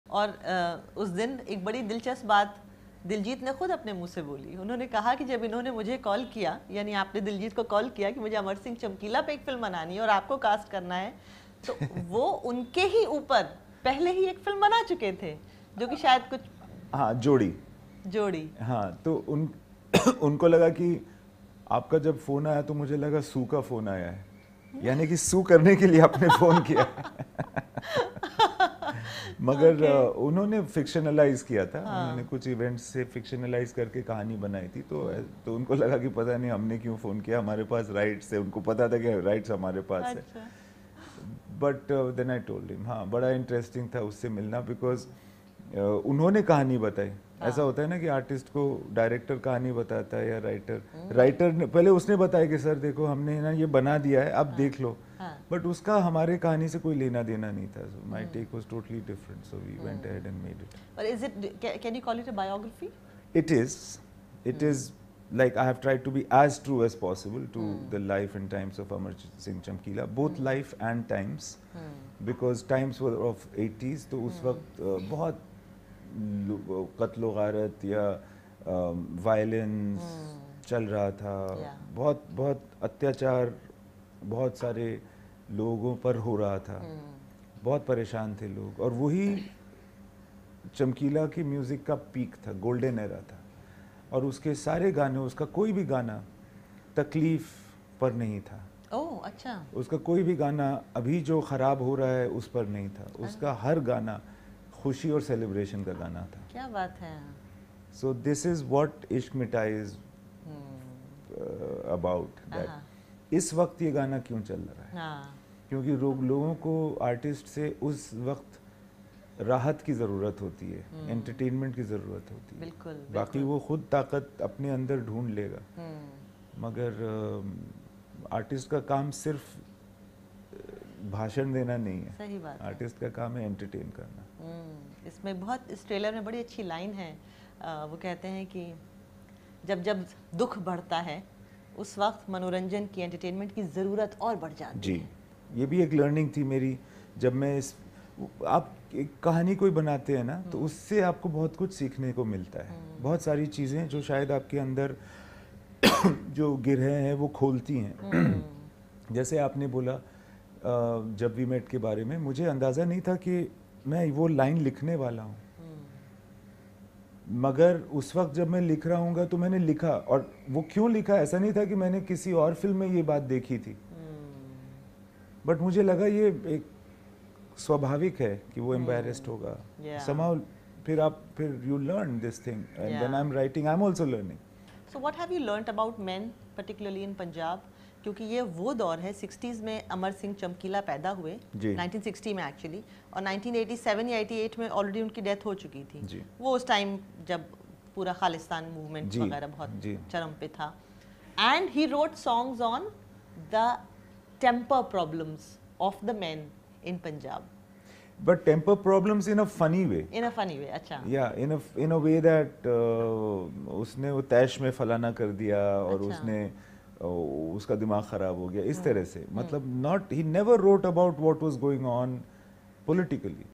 Imtiaz Ali On Amar Singh Chamkila I Diljit Dosanjh | AR Rahman I Exclusive
With many calling the film 'Amar Singh Chamkila' Imtiaz Ali's grand comeback, the director sat down for an exclusive chat with CNN-News18's Atika Farooqui to decode his own craft, as well as Amar Singh Chamkila's to understand what made him so special.